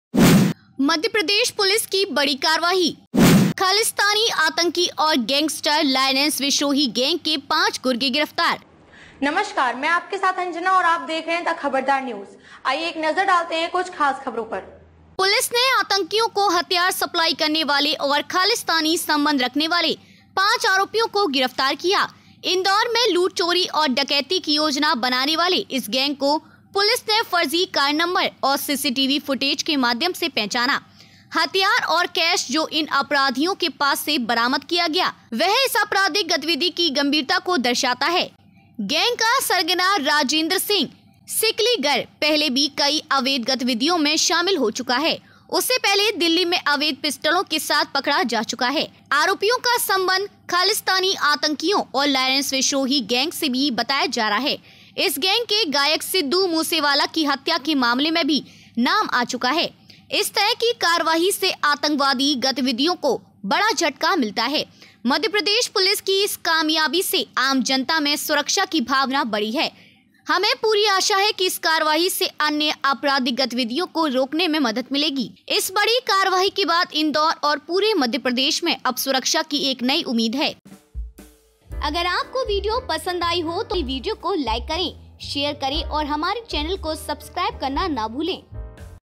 आतंकियों को हथियार सप्लाई करने वाले और खालिस्तानी संबंध रखने वाले पाँच आरोपियों को गिरफ्तार किया।"
पुलिस ने आतंकियों को हथियार सप्लाई करने वाले और खालिस्तानी संबंध रखने वाले पाँच आरोपियों को गिरफ्तार किया।
 "इंदौर में लूट-चोरी और डकैती की योजना बनाने वाले इस गैंग को पुलिस ने फर्जी कार नंबर और सीसीटीवी फुटेज के माध्यम से पहचाना।"
 "हथियार और कैश, जो इन आरोपियों के पास से बरामद किया गया, वह इस अपराधिक गतिविधि की गंभीरता को दर्शाता है।"
  "गैंग का सरगना, राजेंद्र सिंह सिकलीगर, पहले भी कई अवैध गतिविधियों में शामिल हो चुका है। उसे पहले दिल्ली में अवैध पिस्टलों के साथ पकड़ा जा चुका है। इस तरह की कार्रवाई से आतंकवादी गतिविधियों को बड़ा झटका मिलता है
"मध्य प्रदेश पुलिस की इस कामयाबी से आम जनता में सुरक्षा की भावना बढ़ी है।"
"हमें पूरी आशा है कि इस कार्रवाई से अन्य अपराधिक गतिविधियों को रोकने में मदद मिलेगी।"